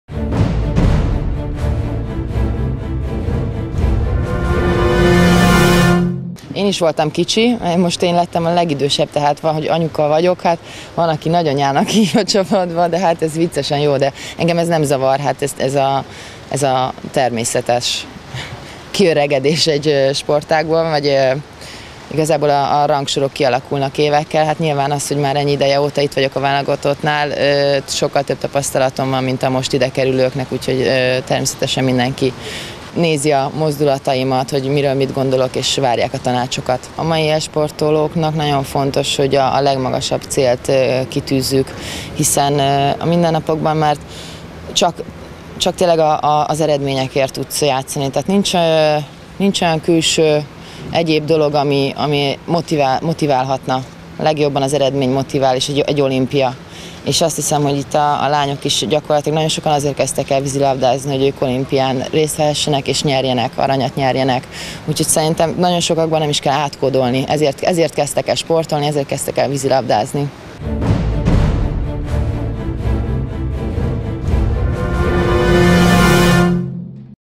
Drávucz Rita: Van, aki nagymamának szólít - Hathárom.hu
Drávucz Rita, a női  vízilabda-válogatott játékosa a Hathárom TV kamerája előtt beszélt a válogatottról és mindannyiuk álmáról, az olimpiai aranyról.